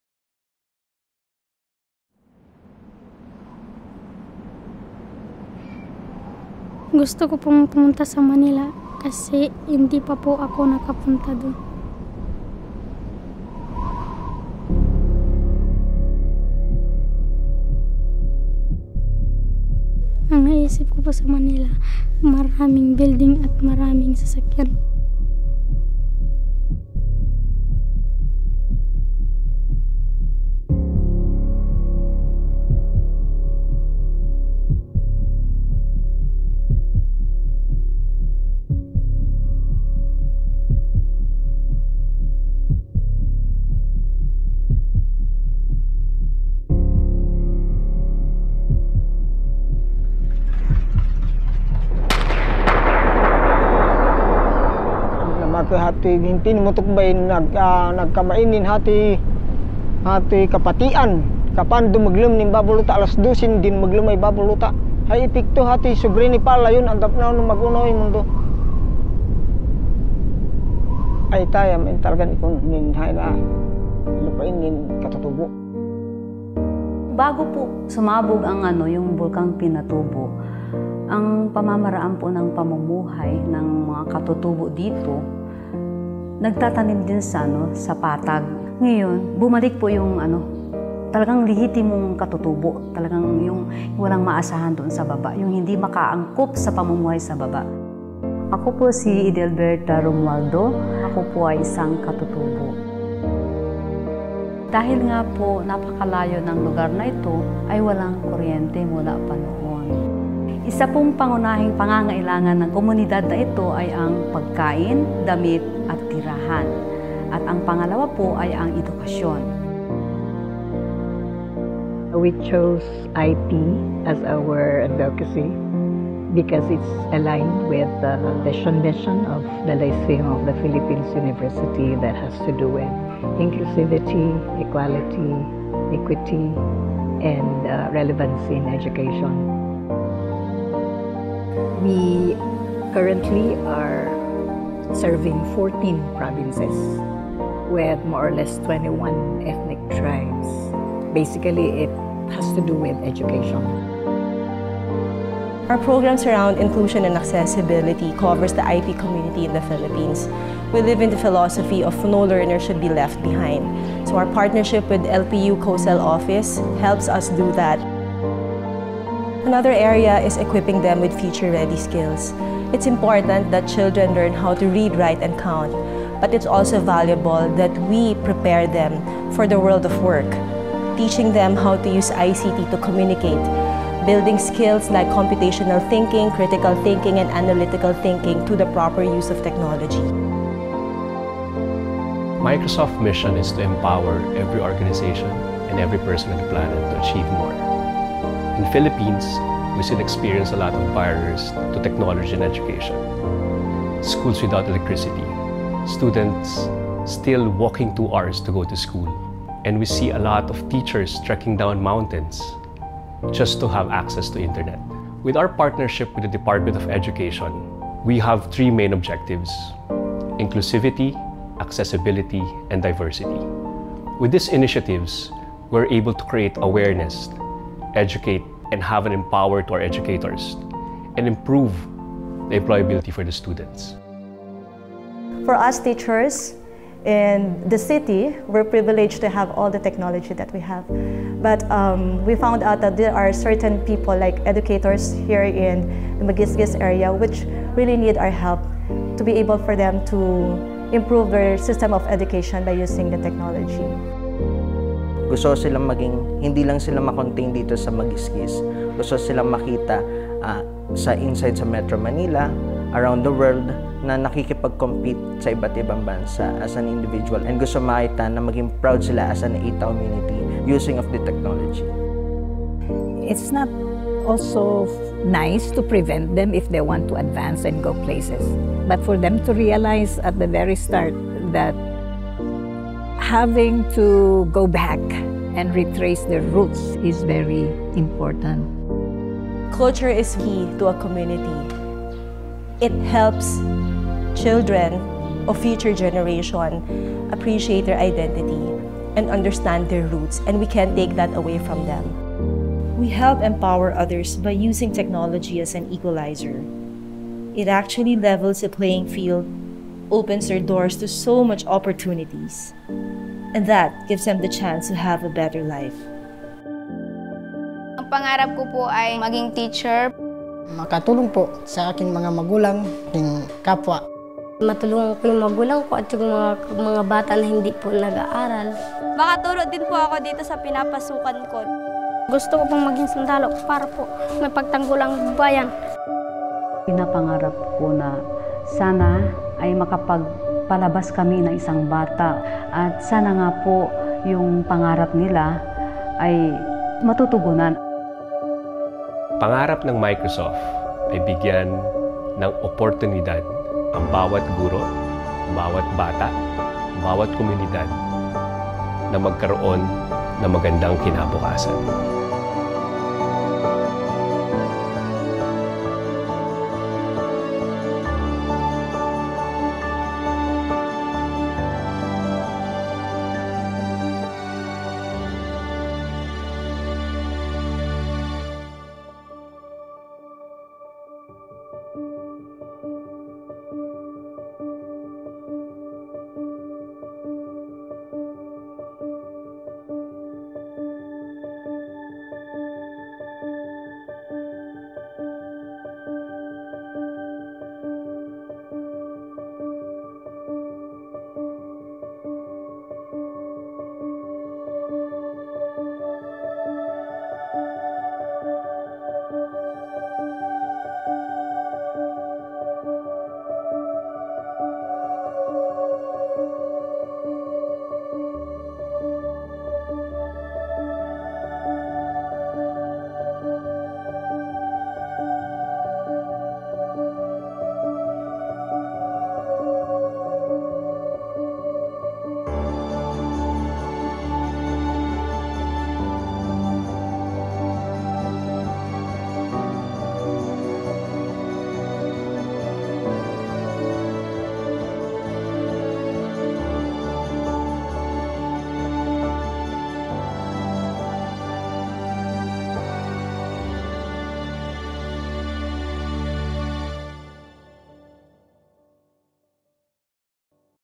Microsoft and Lyceum of the Philippines University
Technology opens a window of opportunity to everyone who uses it. Discover how Microsoft and Lyceum of the Philippines University Manila are empowering the indigenous community in the country.